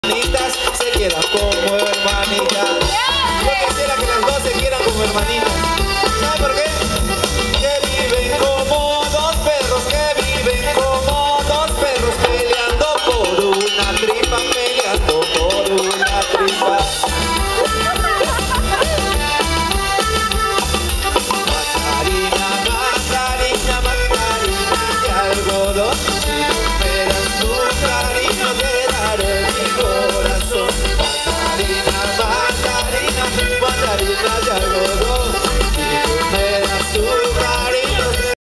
Papillon y sus bailarinas
Fiesta de la peruanidad en Huancayo con la animaciòn de Lizeth Soto.